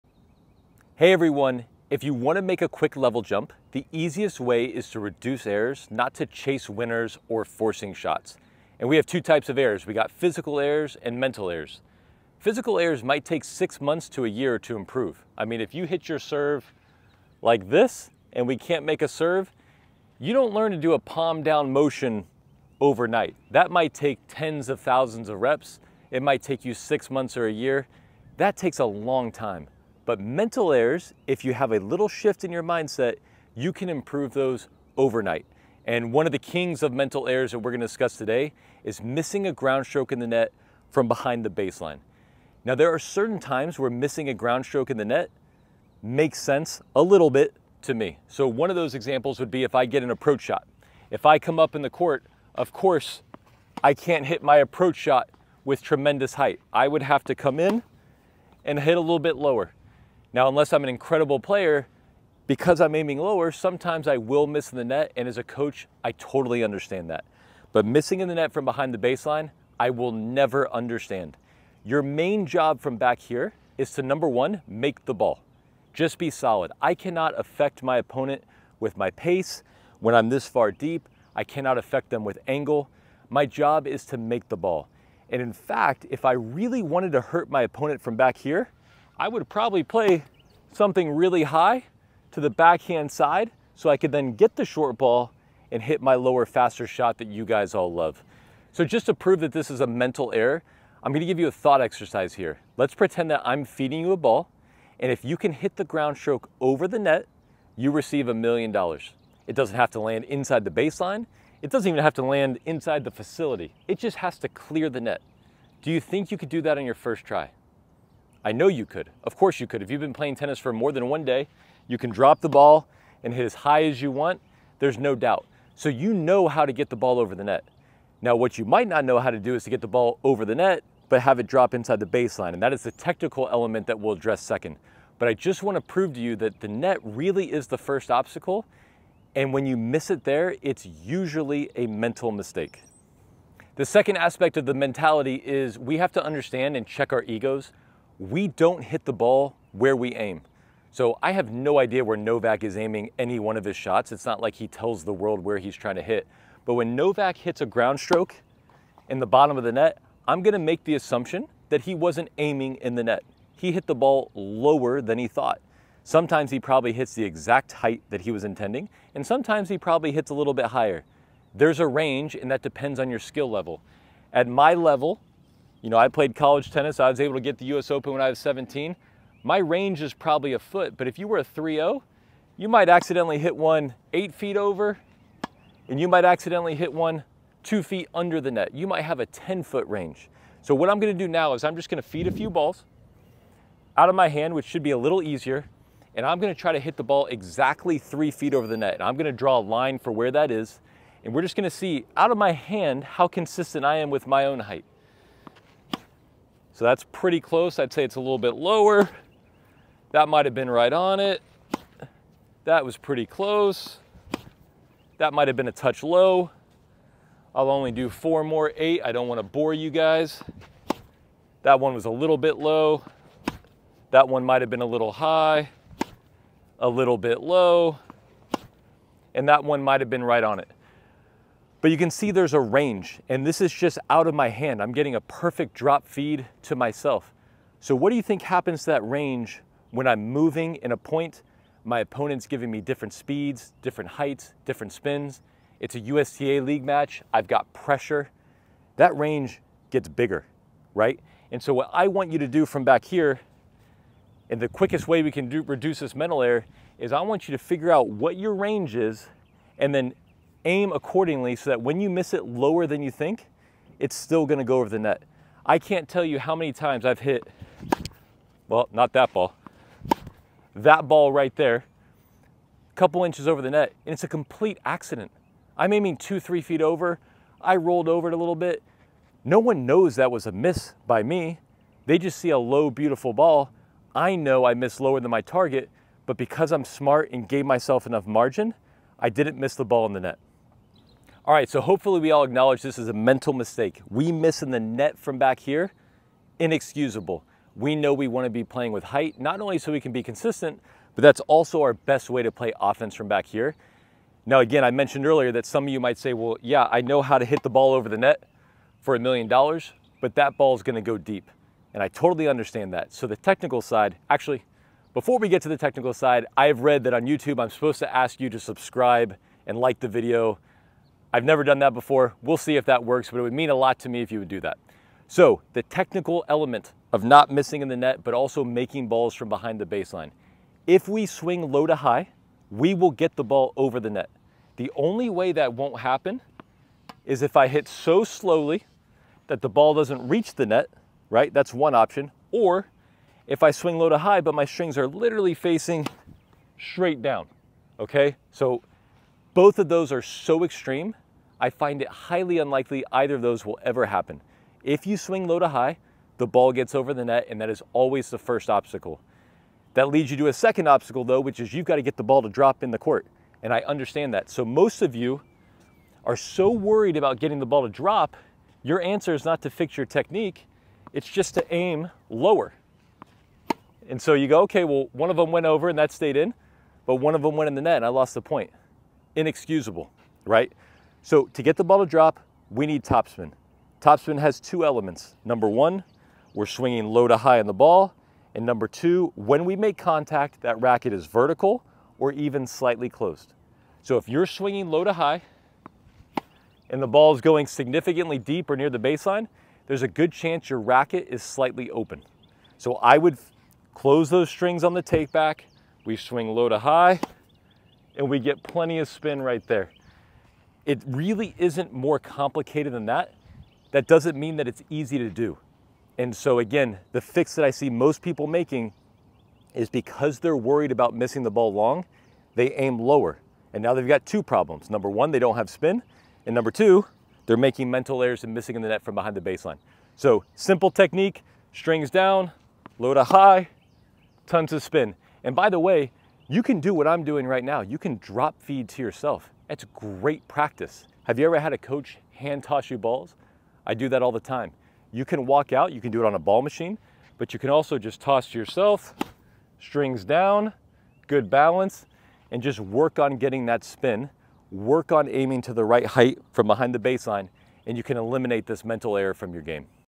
Avoid this MENTAL error from the baseline
There are two main types of errors. Mental errors, and physical errors. Physical errors are understandable. Mental errors are inexcusable.

One of the killer mental errors is missing in the net from behind the baseline. In this video I share my best advice for choosing a target to avoid the net, while also throwing in a few technical tips for lifting the ball above the net.